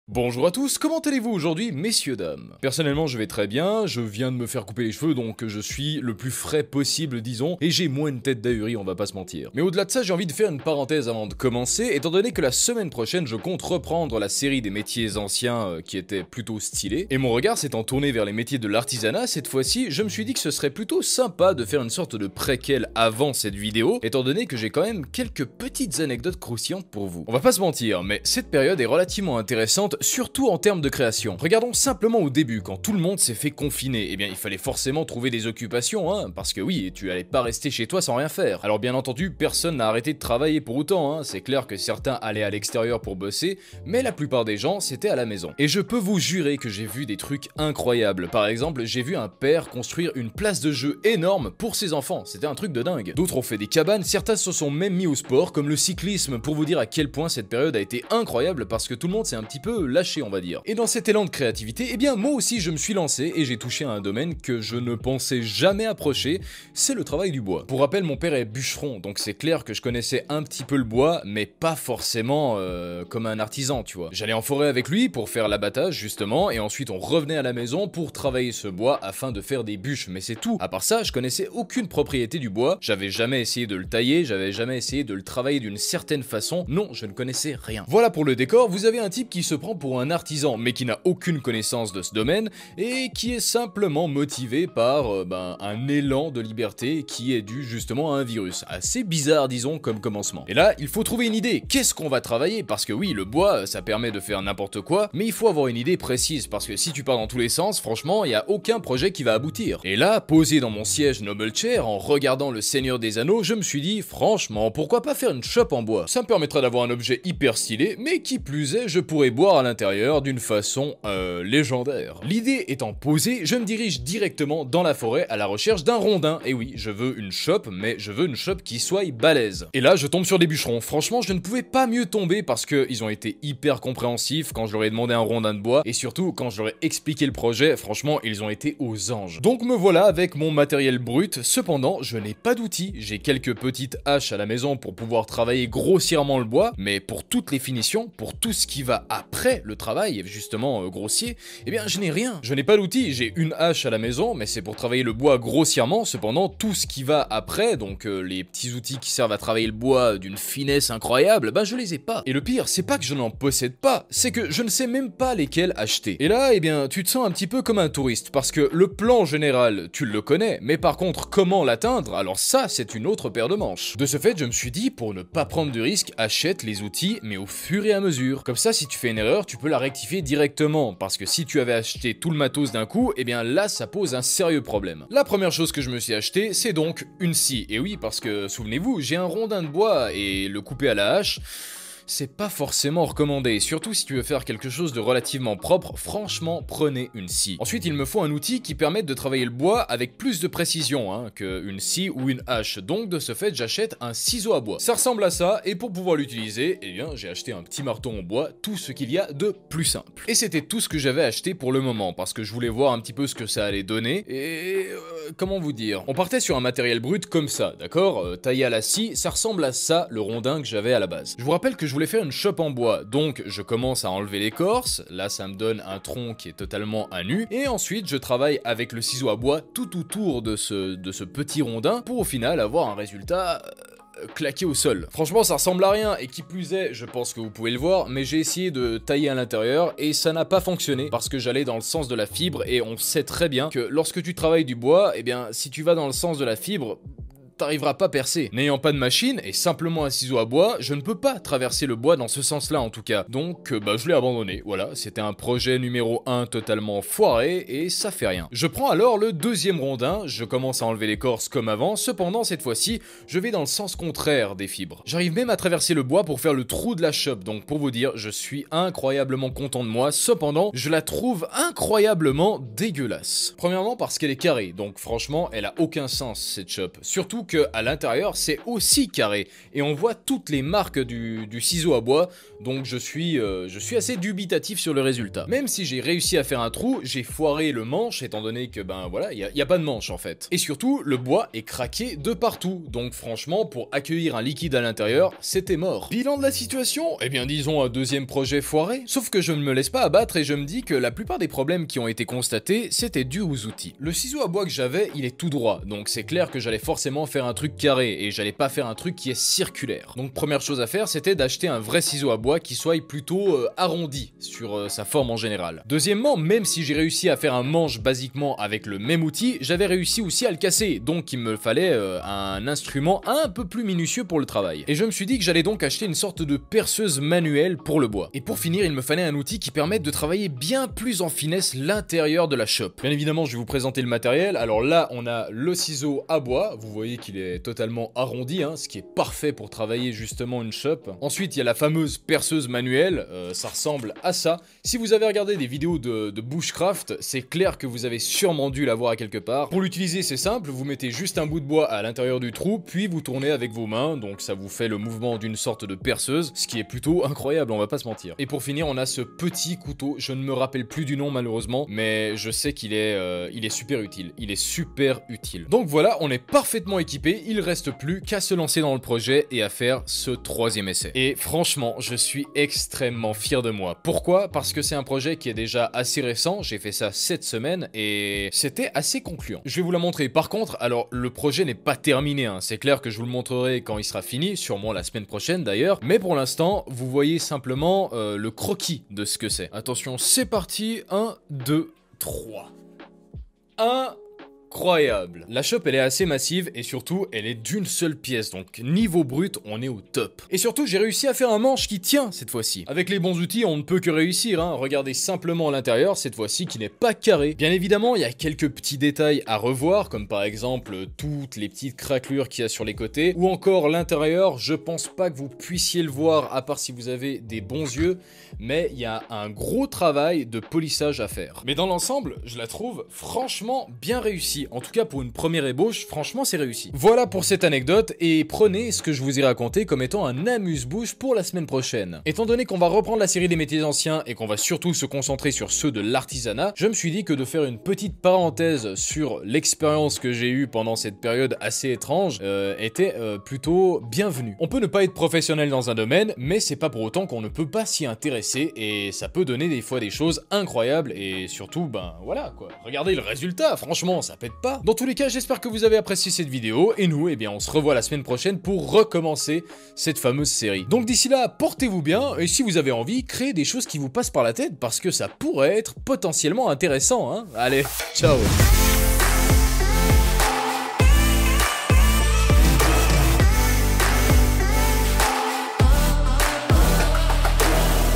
JE FABRIQUE UNE CHOPE EN BOIS!🍺
Cette fois-ci je vous partage l'une de mes occupation pendant cette période compliquée, c'est le travail du bois.